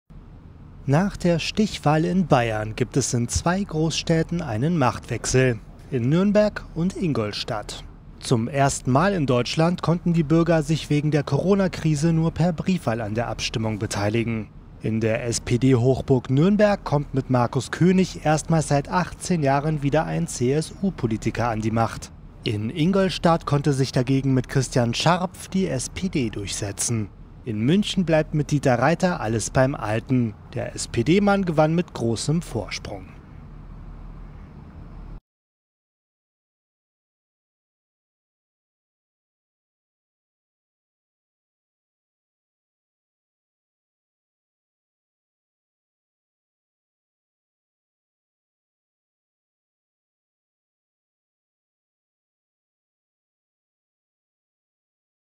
STICHWAHL IN BAYERN: Machtwechsel in zwei Großstädten
Nach der Stichwahl in Bayern gibt es in in den Großstädten Nürnberg und Ingolstadt einen Machtwechsel. Zum ersten Mal in Deutschland konnten die Bürger sich wegen der Corona-Krise nur per Briefwahl an der Abstimmung beteiligen.
In der SPD-Hochburg Nürnberg kommt mit Marcus König erstmals seit 18 Jahren wieder ein CSU-Politiker an die Macht. In Ingolstadt konnte sich dagegen mit Christian Scharpf die SPD durchsetzen. In München bleibt mit Dieter Reiter alles beim Alten. Der SPD-Mann gewann mit großem Vorsprung.